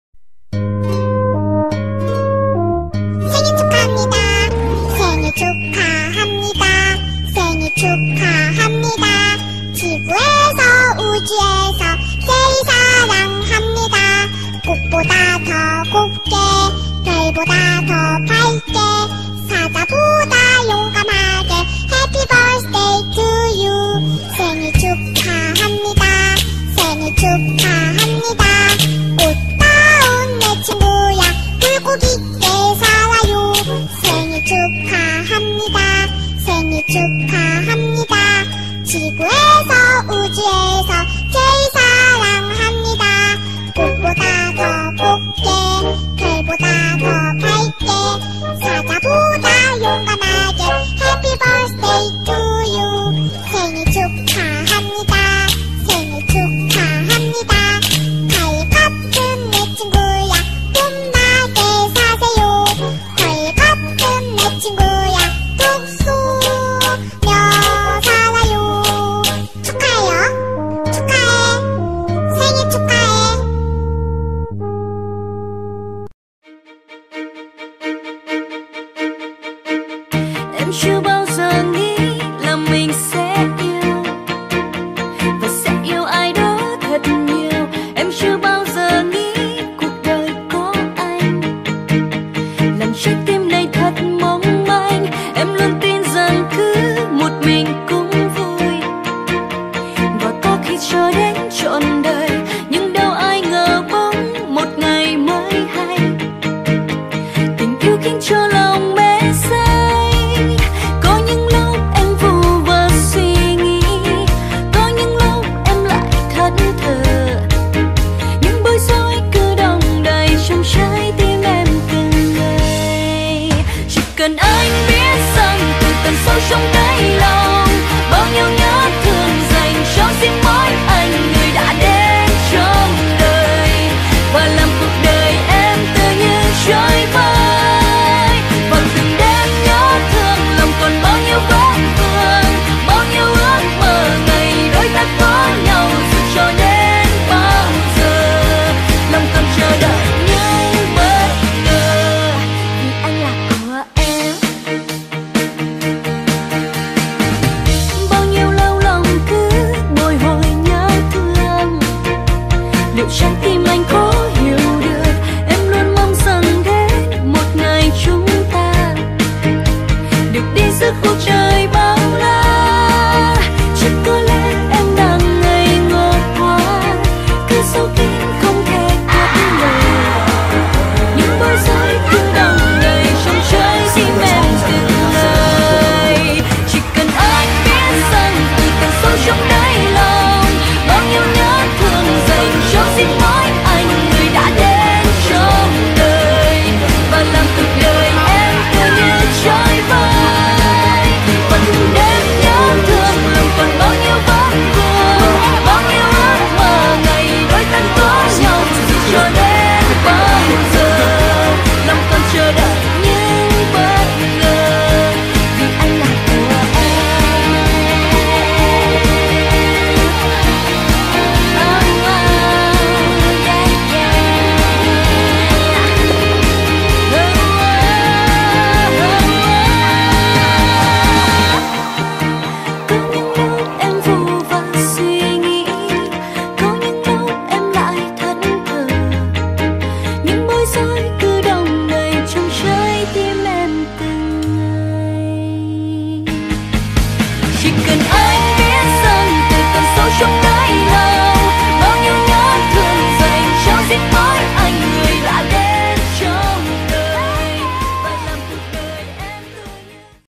Happy birthday Wang Jackson 생일 축하 왕 잭슨 28/3
Happy birthday My JACKSON
I couldn't create a perfect Birthday video for you due to my study schedule is exactly full so my free time even can count like a minute :(
anyway I was anticipating to re-create but, absolutely I couldn't :( I'm sorryyyyyyyyy